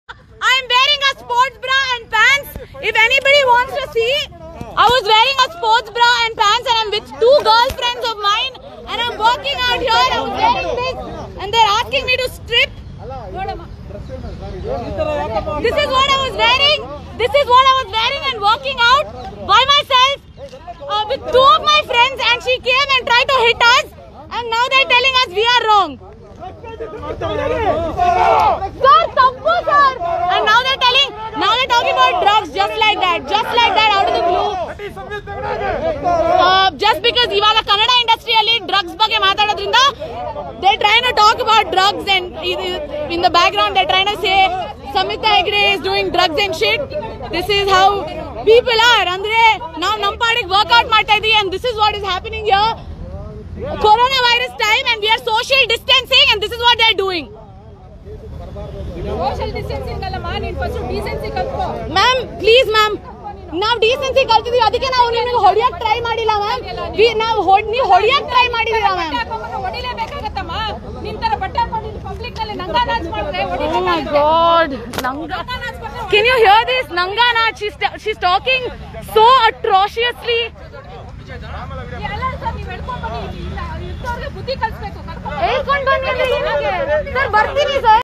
Hola Hooping Karen
Hola Hooper explains what Karen did to her.